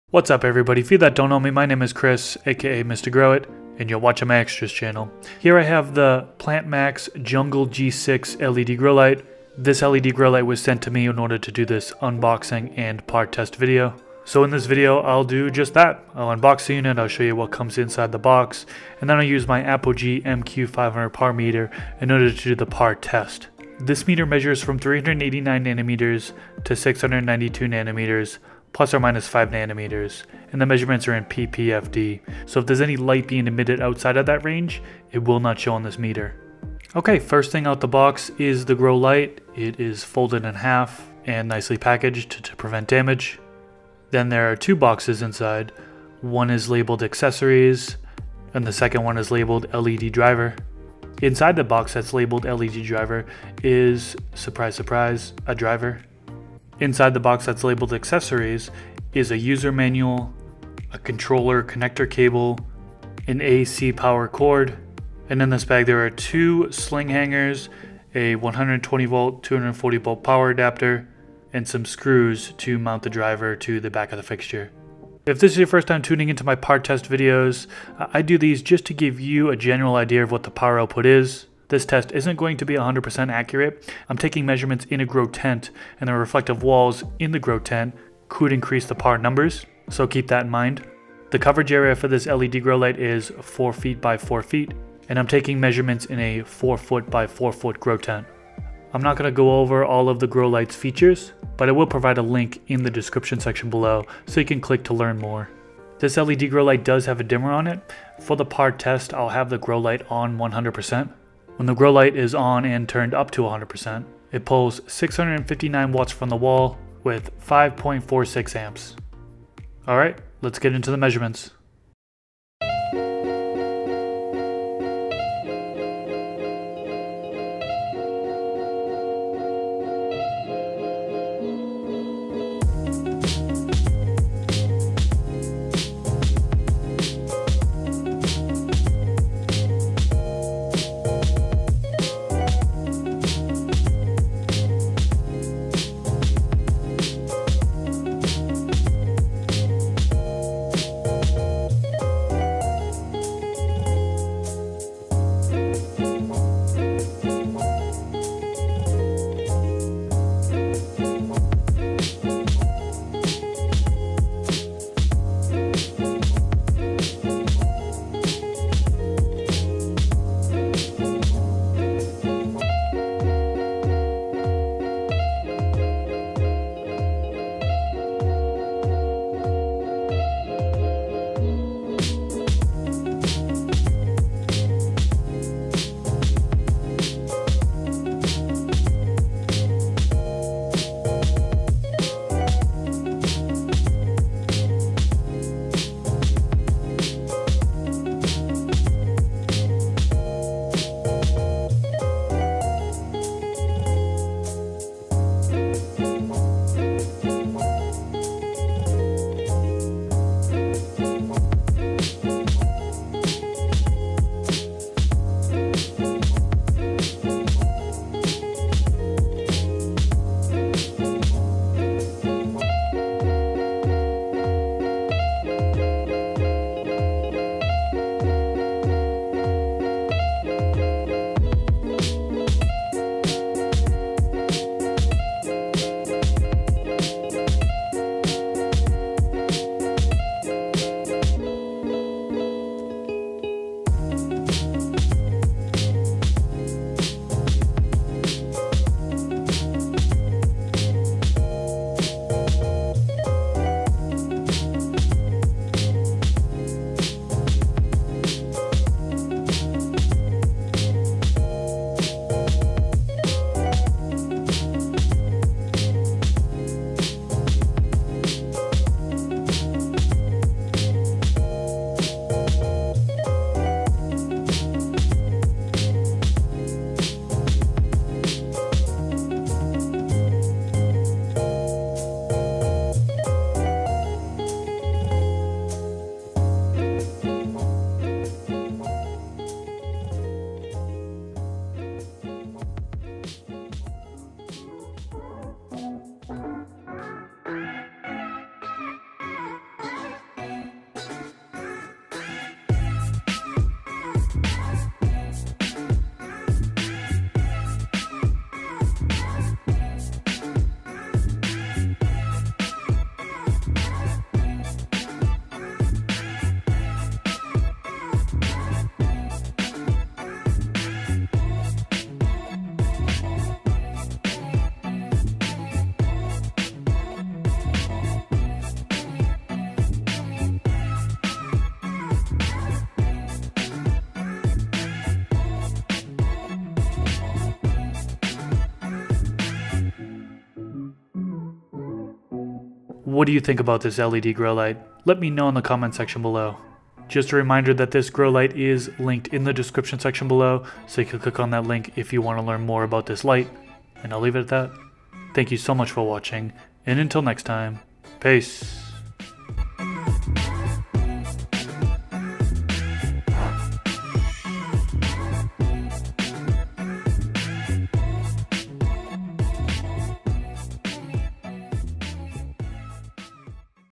4x4 LED Grow Light Unboxing & PAR Test! - PlantMax Jungle G6 LED Grow Light
In this video I unbox and PAR test the Jungle G6 LED Grow Light by PlantMax. What do you think about this 4x4 LED grow light and these PAR measurements?

This gardening video is for educational and/or documentary purposes and isn't designed to help or encourage others to imitate.

PAR meter
Meter wand
Cameras used